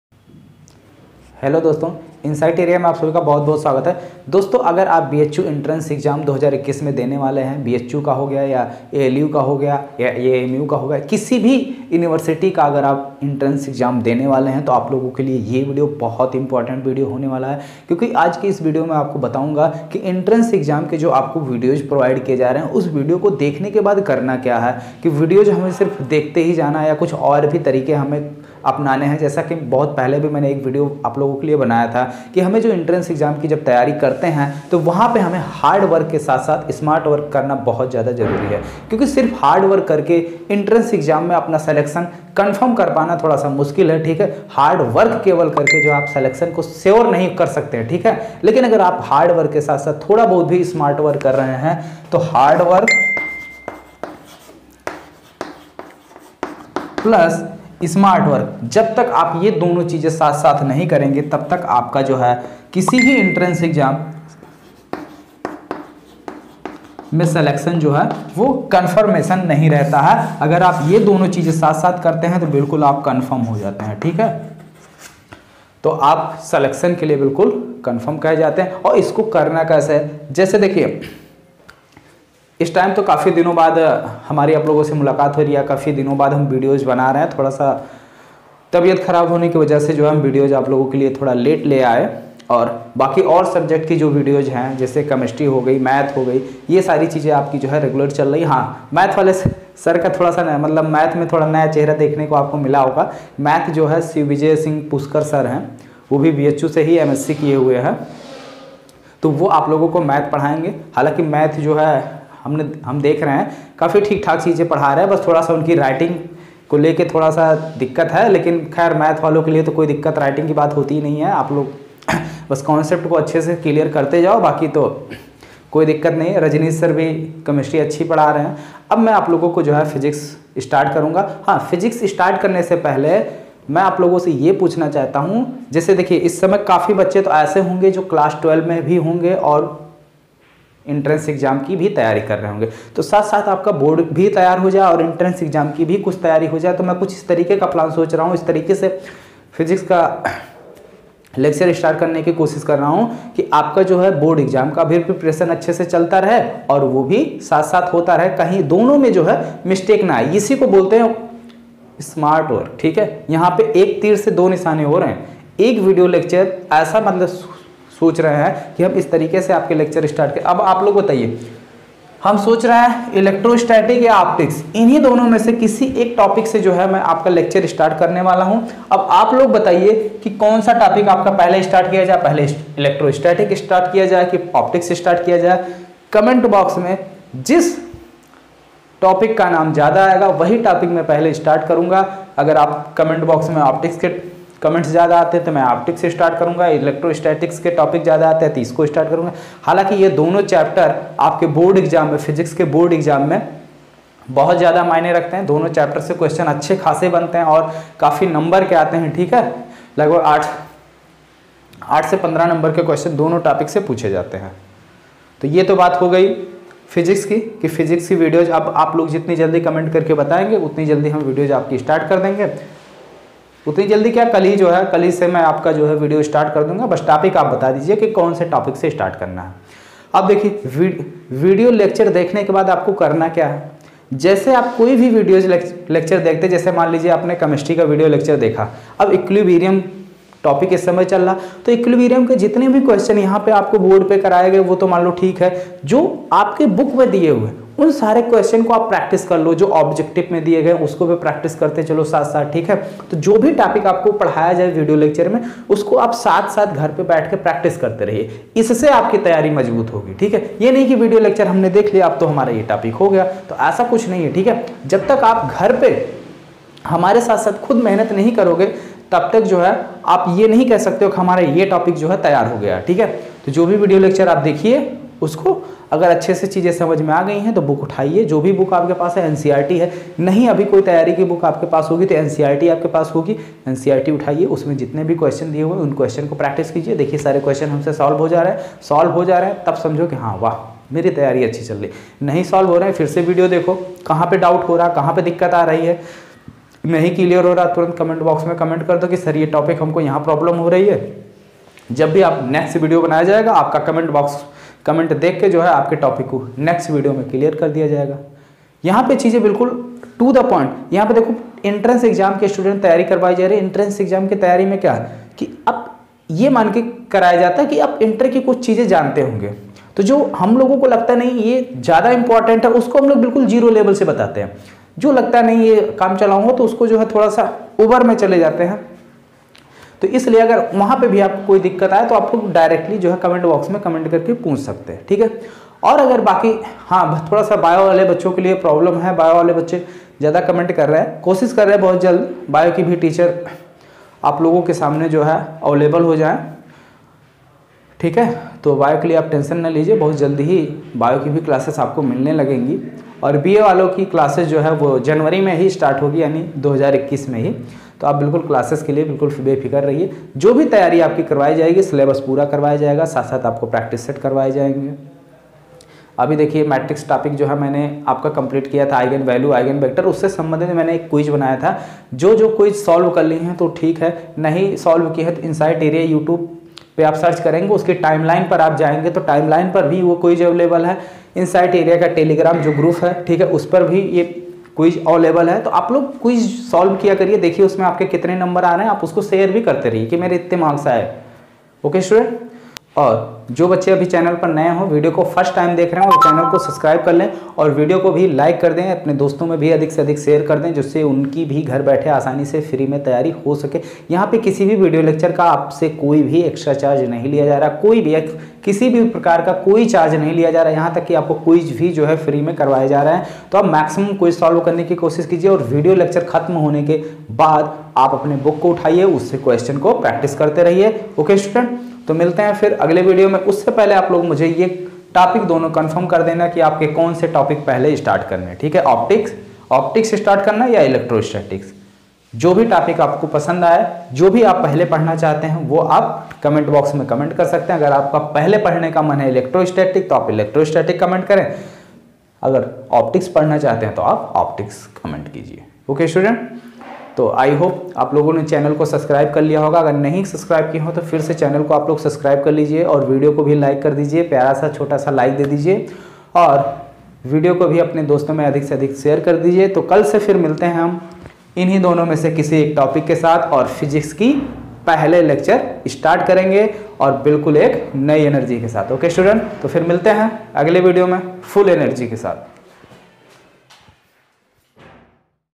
BHU, AMU, AU ENTRANCE EXAM 2021| INTRODUCTION CLASS | PHYSICS CLASS| HOW TO STUDY FOR ENTRANCE EXAM
BHU, AMU, AU ENTRANCE EXAM 2021
INTRODUCTION CLASS
PHYSICS CLASS
HOW TO STUDY FOR ENTRANCE EXAM

This video is very useful for  BHU entrance exam 2021.

Insightatrea.